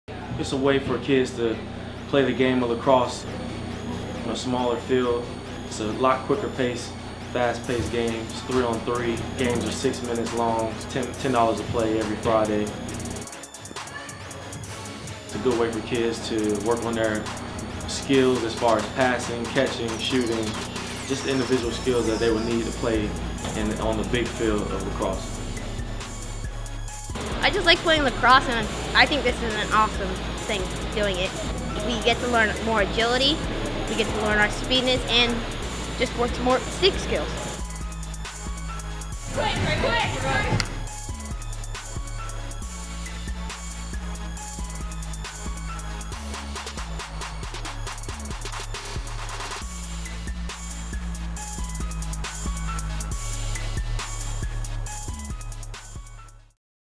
Game Changer Facility - Speed Lacrosse
Promo Video for the Game Changer Facility, located in Winter Park, FL.